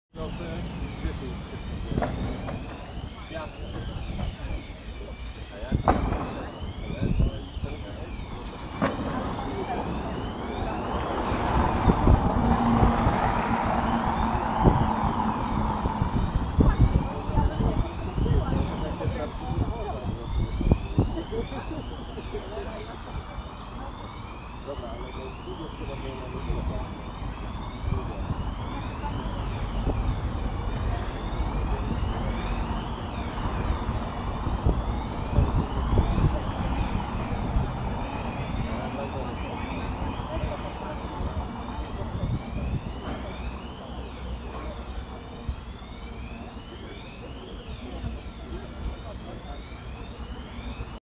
Pożar w Pałacu Prezydenckim ( w jednym ze skrzydeł )
Dziś w jednym z okien Pałacu Namiestnikowskiego pojawił się dym.